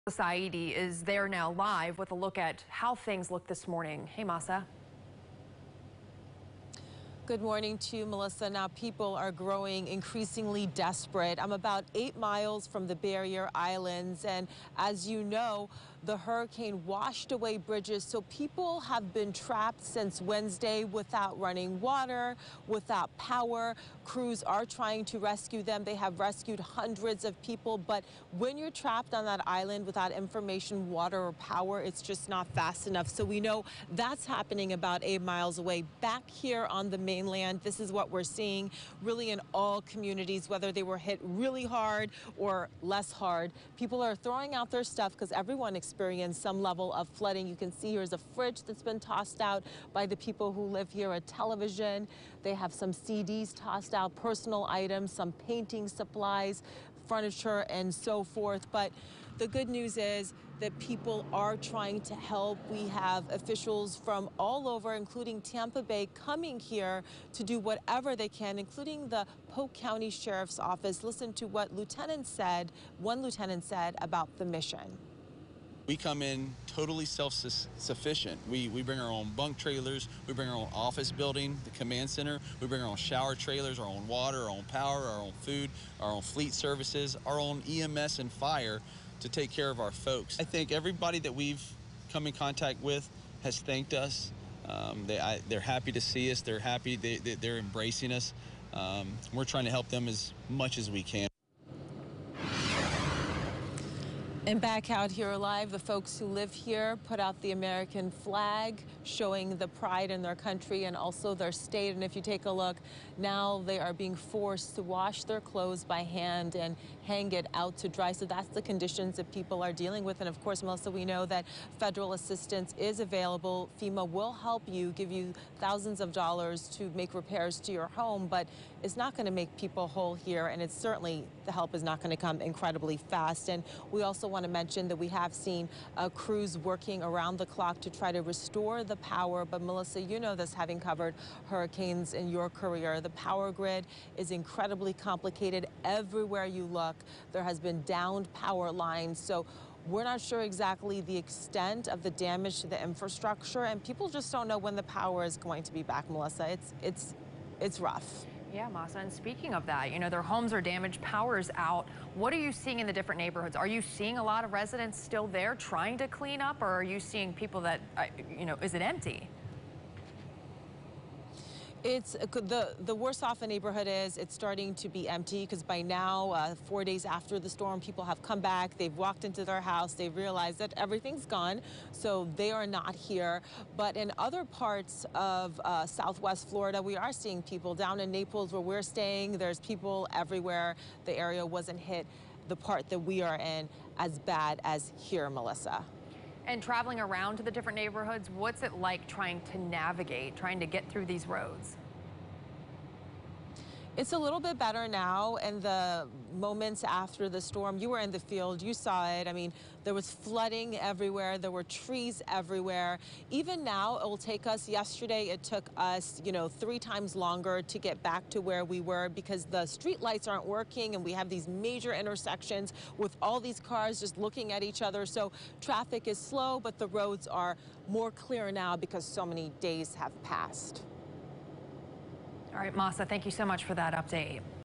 Search and rescue operations continue on barrier islands in SW Florida
8 On Your Side Investigator Mahsa Saeidi has the latest at 7:30am.